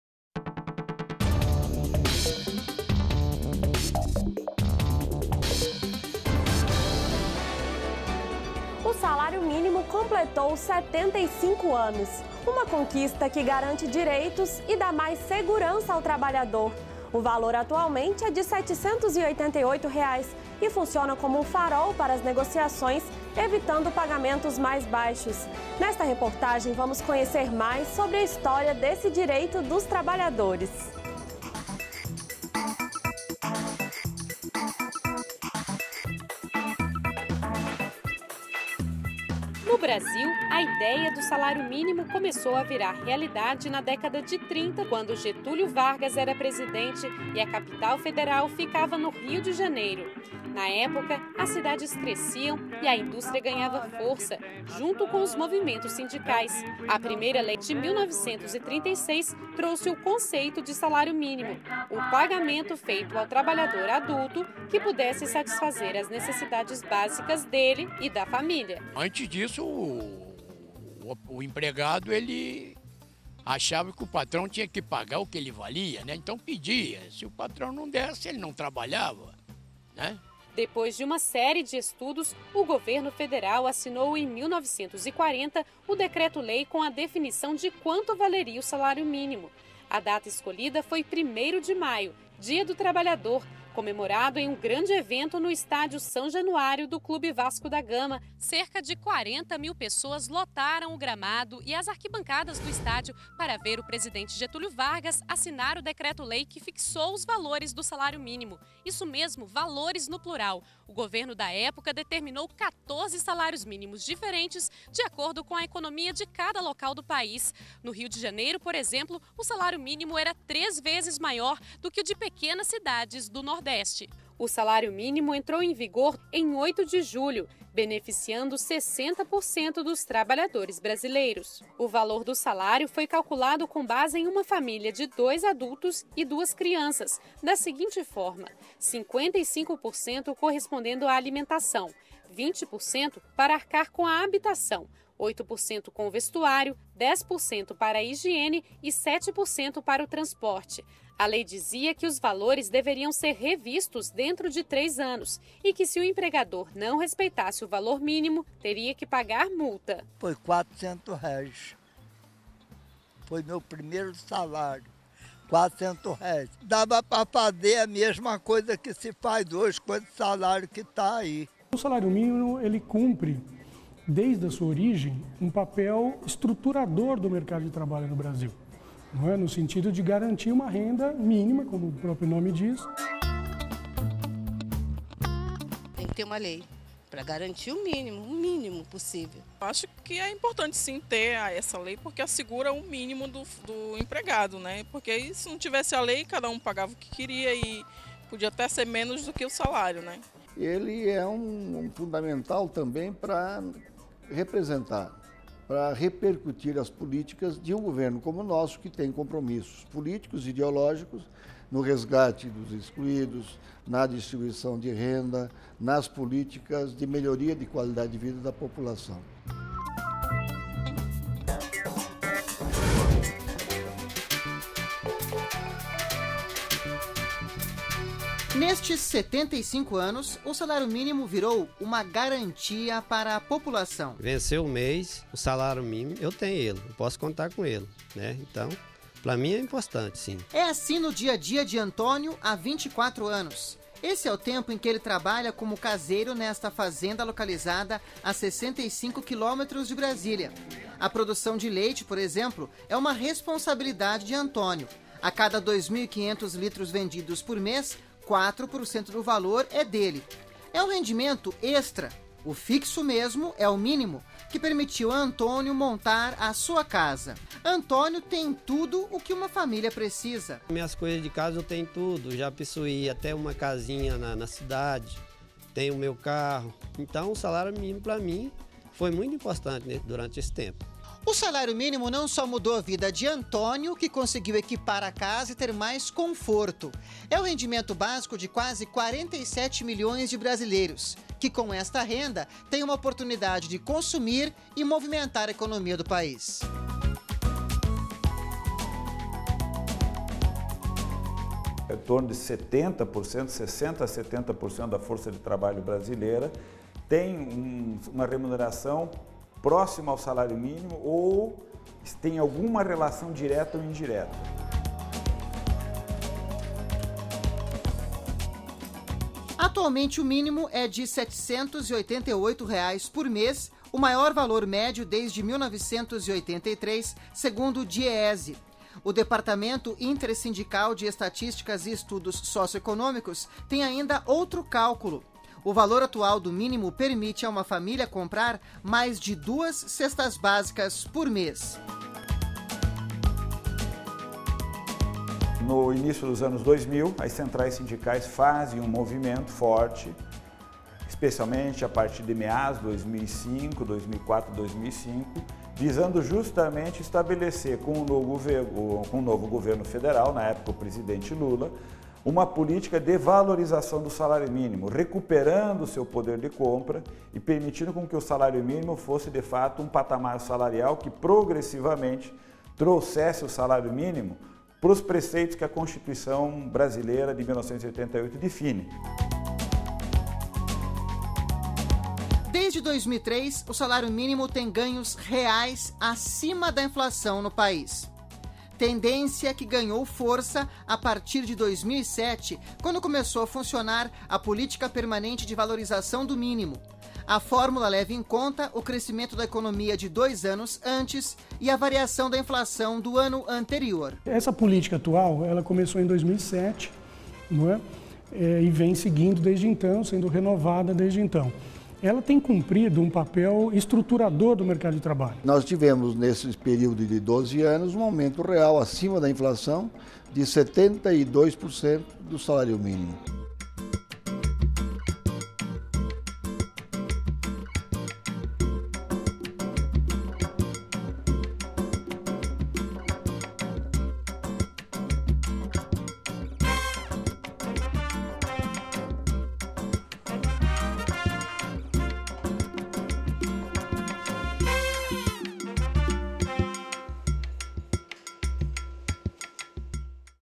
Salário mínimo completa 75 anos com maior valor de compra
DA REDAÇÃO - 17.06.15: Ao completar 75 anos de vigência no Brasil, o salário mínimo registrou o maior valor real desde 1983. Para o trabalhador, o salário é uma conquista,  que garante direitos e mais segurança. Atualmente, o valor do salário minimo é de R$ 788. O salário mínimo deve cobrir as necessidades do trabalhador e de sua família, realizando o poder de compra e levando em consideração o fator fundamental para a redução da desigualdade no país.